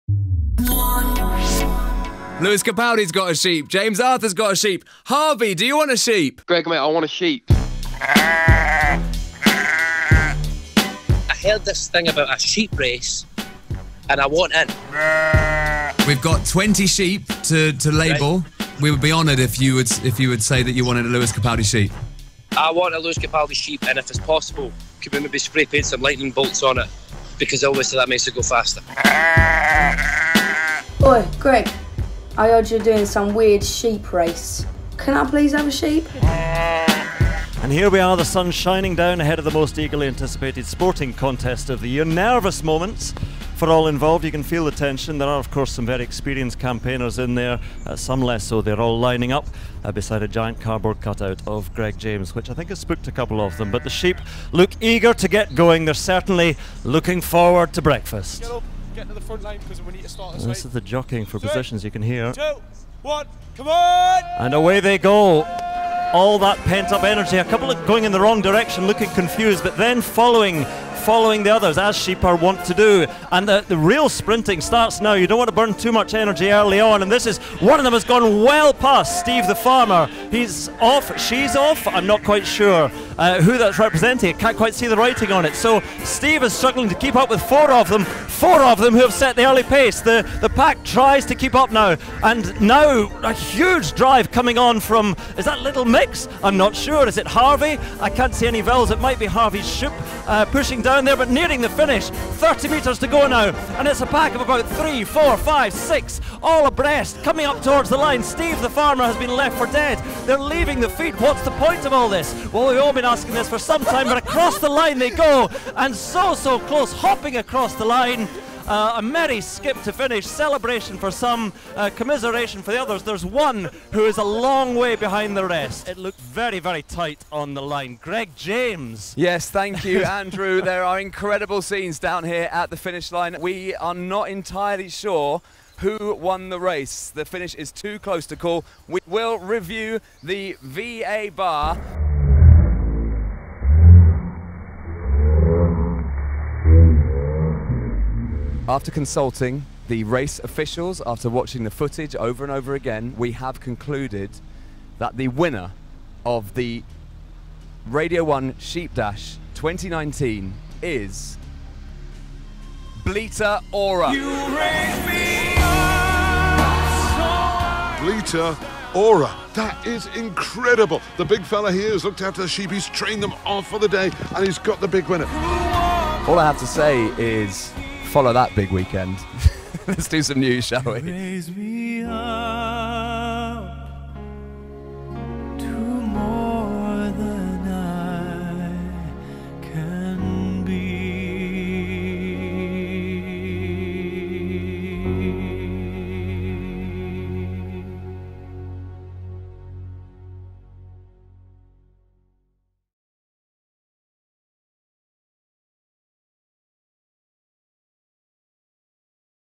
Radio 1's Big Sheep Dash
Ahead of Radio 1's Big Weekend 2019 in Middlesbrough Greg James went to a farmers field to start a big sheep dash!

- We’ve taken great care to ensure our sheep are safe and happy during the dash

- Experts have been contacted and we’ve taken their advice on how to ensure the sheep are happy

- The dash is taking place at the time the sheep would ordinarily be eating their breakfast, so we are fitting in with their schedule